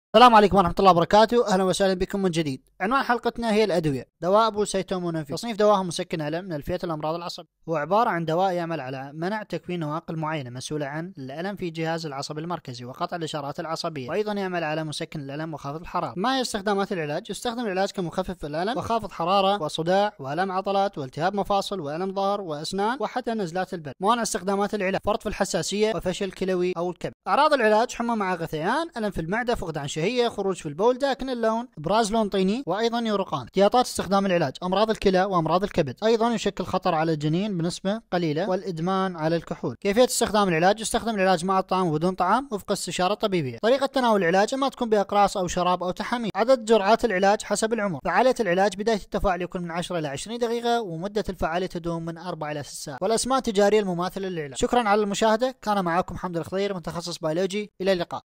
ماهو دواء ابو - اسيتامينوفين Acetaminophen | صيدلية
كورس كامل عن صيدلة وخاص بالادوية مجانا .
يقدم حلقات عديدة في مجال الطب والعلوم.

اعداد والتقديم : أ.بايولوجي – حامد الخضير

الحلقة الاولى ابو - اسيتامينوفين Acetaminophen.
 تم ايجاز النقاط المهمة في الفديو كاملاً.
دمتم بخير و عافاكم الله من الأمراض .

@ hamidalkhudahir

hamidalkhudhair?fs=e&s=cl

hamidalkhuhair

لاتنسى المشاركة و التعليق و الاعجاب بالفديو وشكراً